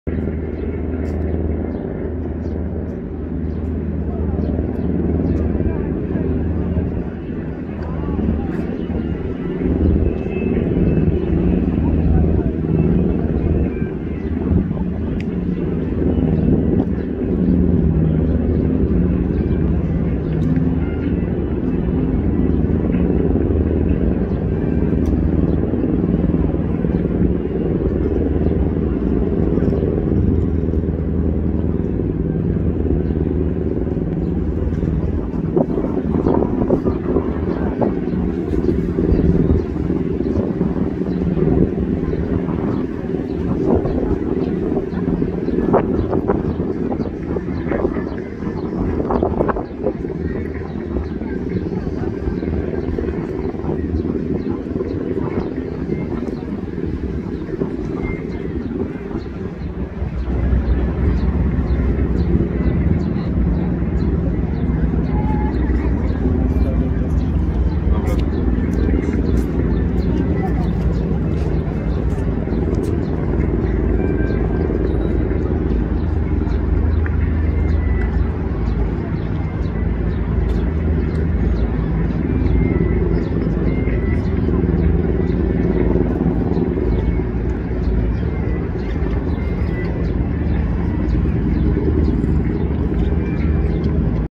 ЧП на Воробъёвых горах. Люди зависли на атракционе. Zipline.
Любители острых ощущений зависли на атракционе Zipline, их спасают сотрудники скай парка на Воробъевых хорах.